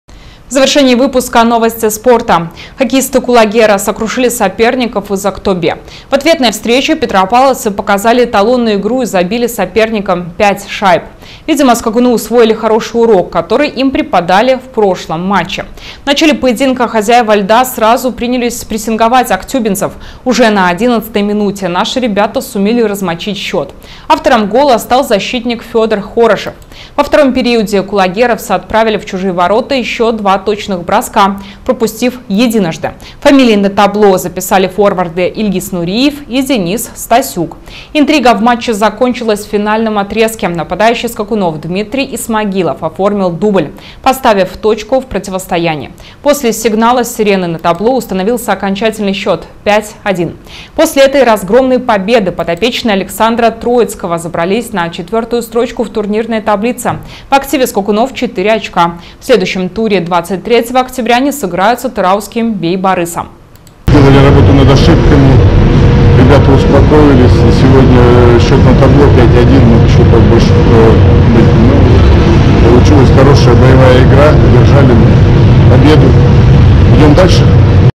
Хоккеисты «Кулагера» сокрушили соперников из «Актобе»
Хоккеисты "Кулагера" сокрушили соперников из "Актобе". В ответной встрече петропавловцы показали эталонную игру и забили соперникам 5 шайб. Видимо, скакуны усвоили хороший урок, который им преподали в прошлом матче.